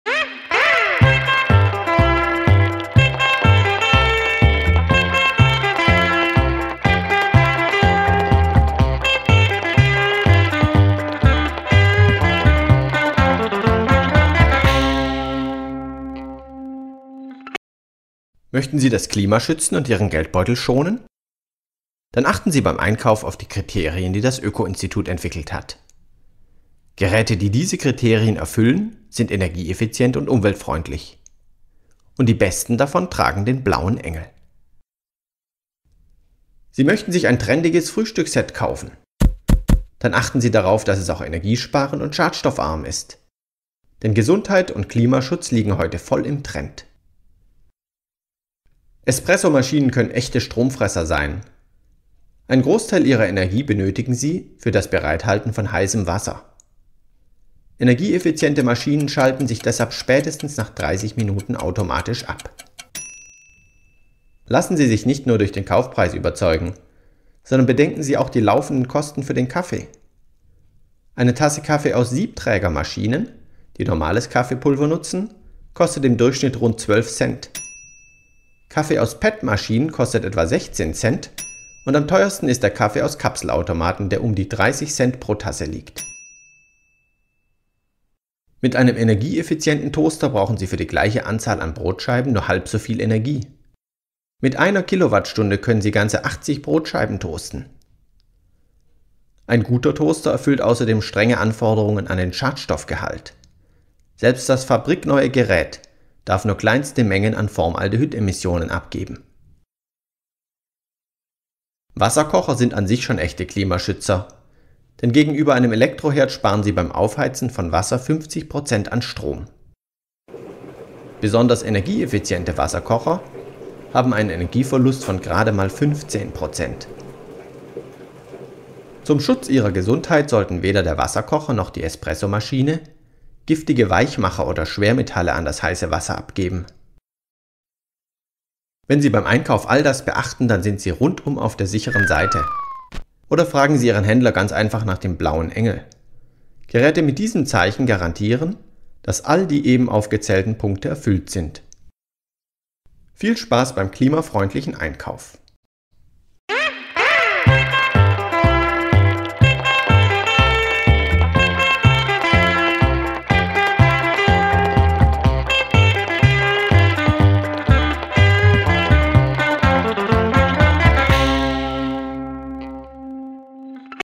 Klimaschutz-Einkaufhilfe für das Frühstücksset
Worauf sollte man achten, wenn man schon beim Frühstück das Klima schützen will? Ob Toaster, Espresso-Maschine oder Wasserkocher -- das Öko-Institut hat Kriterien für klimafreundliche Haushaltsgeräte entwickelt, die auch zur Vergabe des Umweltzeichens der Blaue Engel verwendet werden. Wer nicht den Forschungsbericht lesen möchte, bekommt hier die wichtigsten Ergebnisse in zweieinhalb Minuten.